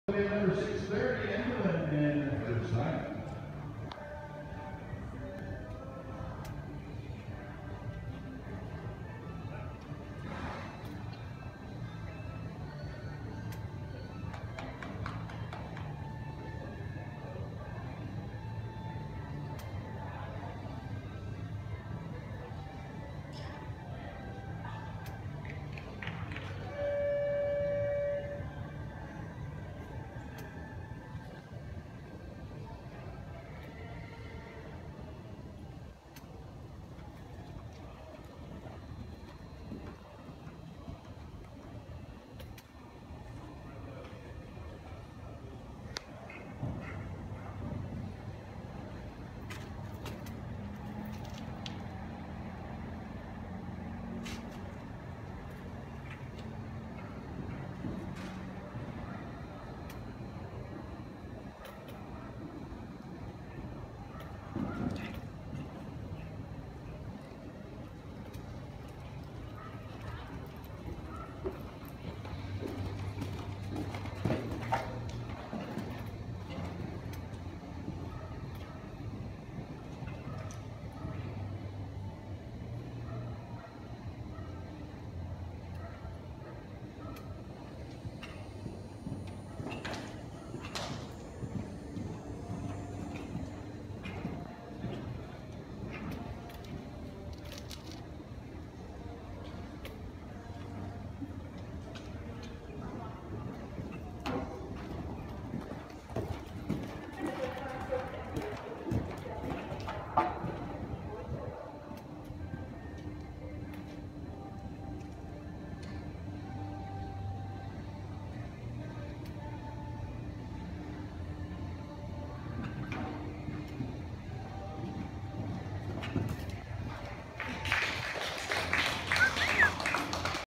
Andolin, Welcome Stake
2005 16h KWPN Gelding
Top money earner for professionals that's now winning for a jr/am. Fast, careful, and very brave. Miles through 1.45m, tons of success in the 1.30m classes and recent Ch/AA wins.